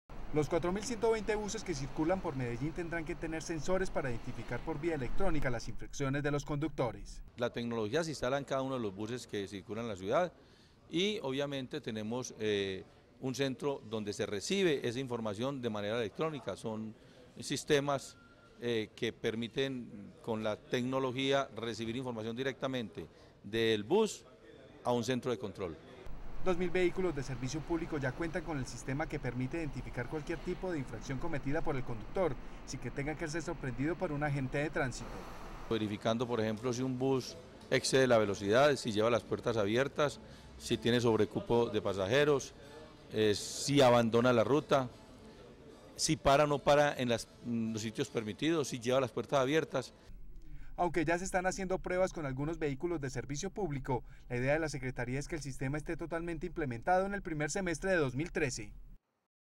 A partir de 2013 los buses serán vigilados vía electrónica
Los conductores que cometan infracciones, serán sancionados con comparendos electrónicos.